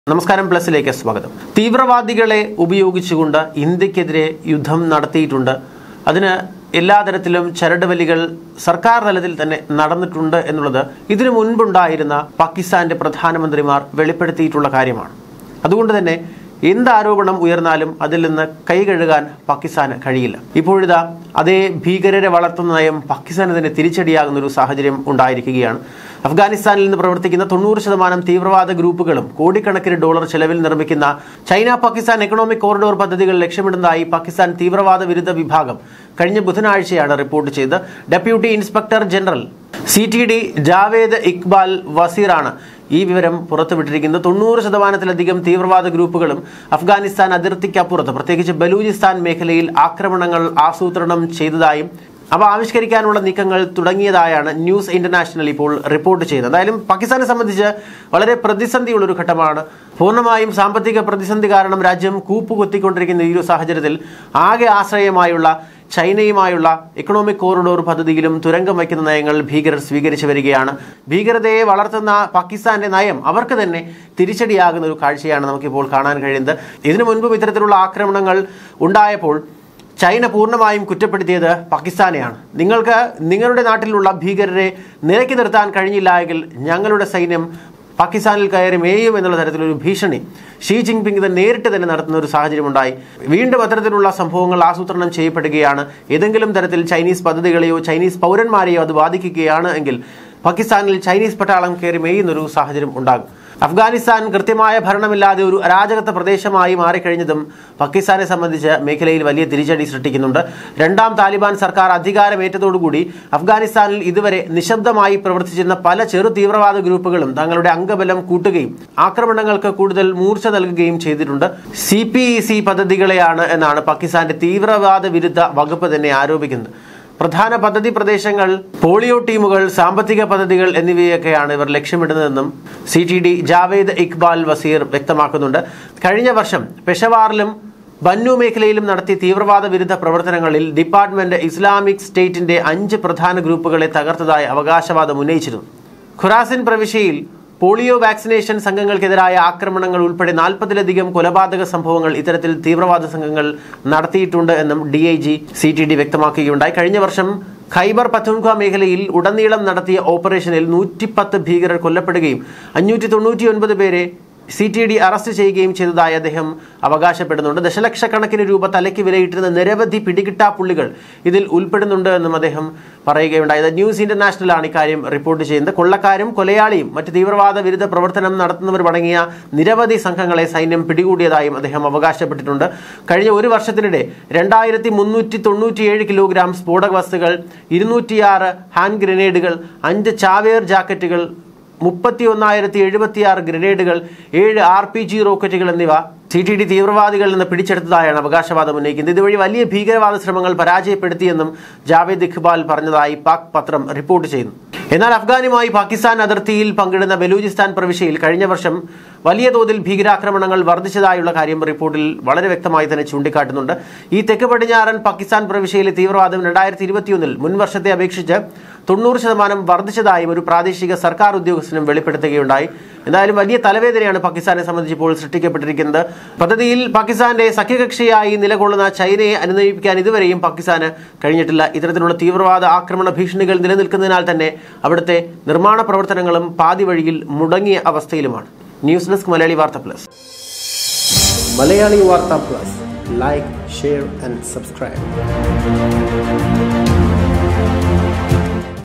Imran Khan അരാജക പ്രദേശമായി മാറി
കഴിഞ്ഞ വര്‍ഷം ഖൈബര്‍ പഖ്തൂണ്‍ഖ്വ മേഖലയില്‍  ഉടനീളം നടത്തിയ ഓപ്പറേഷനുകളില്‍ 110 ഭീകരര്‍ കൊല്ലപ്പെടുകയും 599 പേരെ സിടിഡി അറസ്റ്റ് ചെയ്യുകയും ചെയ്തതായി അദ്ദേഹം അവകാശപ്പെട്ടു. ദശലക്ഷക്കണക്കിന് രൂപ തലയ്ക്ക് വിലയിട്ടിരുന്ന നിരവധി പിടികിട്ടാപ്പുള്ളികള്‍  ഇതില്‍ ഉള്‍പ്പെടുന്നെന്നും അദ്ദേഹം കൂട്ടിചേര്‍ത്തതായി ദ ന്യൂസ് ഇന്റര്‍നാഷണല്‍ റിപ്പോര്‍ട്ട് ചെയ്തു.